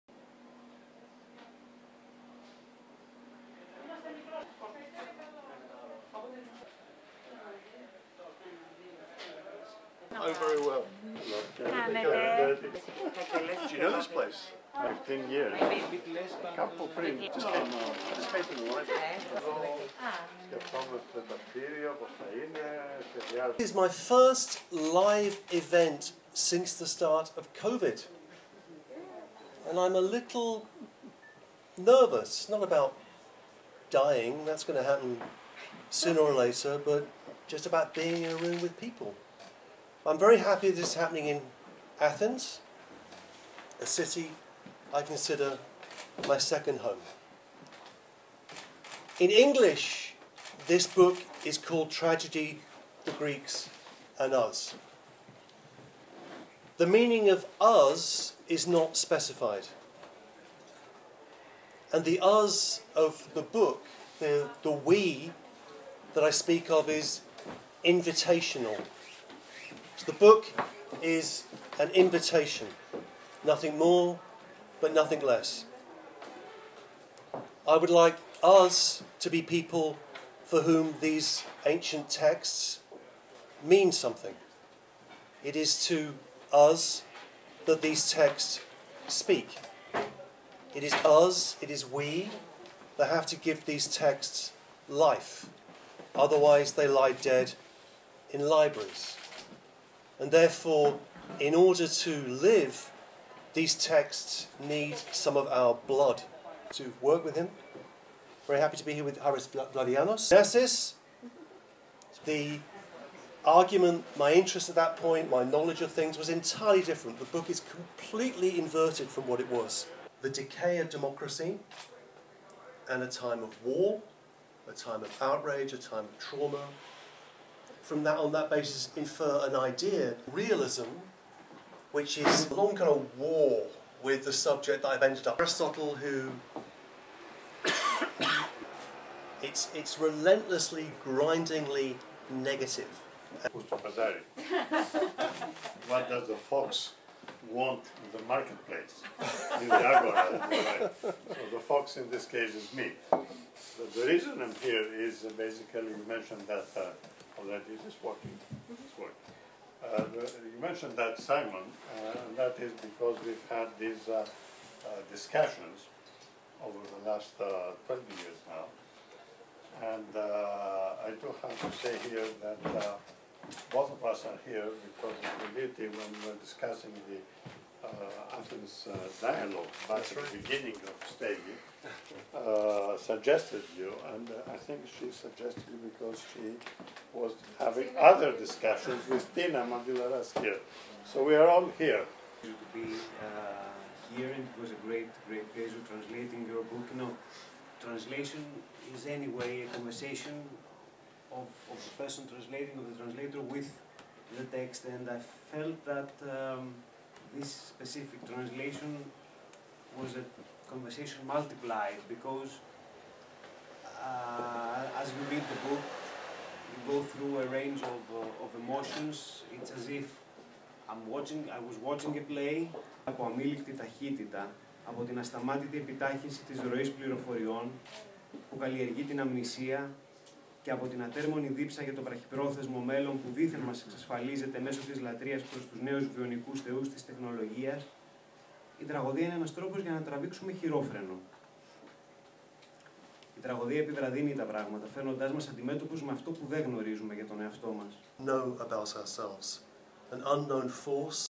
Παρουσίαση του βιβλίου του φιλόσοφου SIMONE CRITCHLEY
Ο καθηγητής, φιλόσοφος και μέλος του ΔΣ του Ιδρύματος Ωνάση, Simon Critchley, παρουσίασε το νέο του βιβλίο του «Η τραγωδία, oι αρχαίοι Έλληνες κι εμείς», που κυκλοφορεί από τις εκδόσεις Πατάκη, στον χώρο του Free Thinking Zone, στην Αθήνα, τη Δευτέρα 8 Νοεμβρίου 2021.  Κοντά του βρέθηκαν ο Αντώνης Παπαδημητρίου, πρόεδρος του Ιδρύματος Ωνάση, αλλά και ο μεταφραστής του, Γιάννης Δούκας. ΑΠΕ ΜΠΕ/ ΑΠΕ ΜΠΕ/ ΚΩΣΤΑΣ ΣΟΡΟΒΟΣ